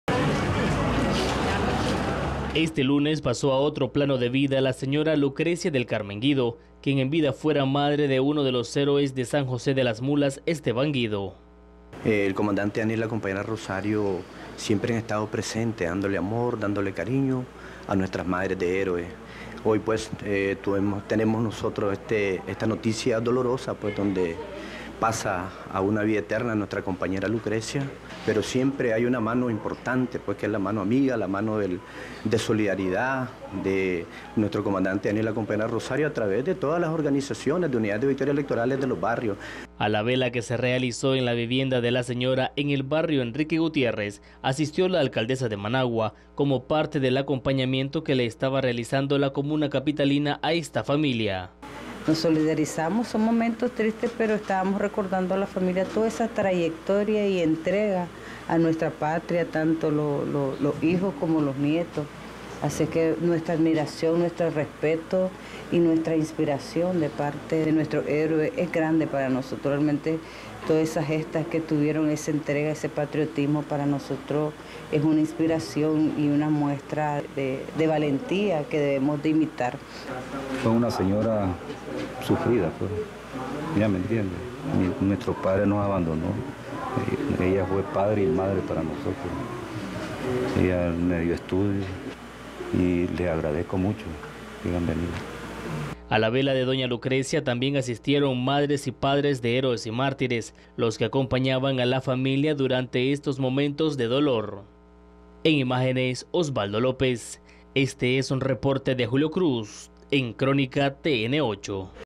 Fallece Lucrecia del Carmen Guido, madre de héroe de San José de Las Mulas - Nicaragua
Este lunes 25 de julio, pasó a otro plano de vida la señora Lucrecia del Carmen Guido, quien en vida fuera madre de uno de los héroes de San José de Las Mulas, Esteban Guido.

“Este acompañamiento no solamente se hace en estos momentos, el comandante Daniel y la compañera Rosario siempre ha estado presente dándole amor dándole y cariño a nuestras madres de héroes. Hoy, pues tenemos nosotros este esta noticia dolorosa, la comunidad le tenía gran aprecio a nuestra compañera, una compañera consciente que la revolución del frente sandinista era necesaria y que teníamos que defenderla ella dio su hijo para que abonar a esta tierra, esta tierra bendita, esta tierra fértil que hoy ya está germinando y está dando fruto”, dijo el secretario político del barrio Luis Enrique Lagos Valladares.

A la vela que se realzó en la vivienda de la señora, en el barrio Enrique Gutiérrez, asistió la alcaldesa de Managua; como parte del acompañamiento que le estaba realizando la comuna capitalina a esta familia.

“La atención a doña Lucrecia siempre estuvo llena de amor, recordemos que con múltiples enfermedades crónica típicas por la edad. Nosotros venimos con esa tensión permanente a través de ese programa hermoso que tiene nuestro buen gobierno que los médicos; nuestras enfermeras, nuestra salud lleguen a cada uno de los hogares. Hoy estamos con la familia trayéndoles ese abrazo de solidaridad y acompañamiento permanente; solidarizamos en este momento triste”, recalcó Reyna Rueda, alcaldesa de Managua.

Atesorar esa muestra del héroe de San José de Las Mulas
Rueda agregó que debemos de también conservar, atesorar esa muestra que han tenido como este ejemplo de valentía de parte de todos los mártires; pero sobre todo de las madres, que han sabido ganarse ese respeto, ese apoyo que viene de cada uno de nosotros.

“Fue una señora sufrida, nuestro padre nos abandonó, ella nos dio estudio y le estoy agradecido”; dijo entre lágrimas el señor Juan Sáenz Guido, hijo de doña Lucrecia.

A la vela de doña Lucrecia asistieron madres y padres de héroes y mártires, los que acompañaban a la familia durante estos momentos de dolor.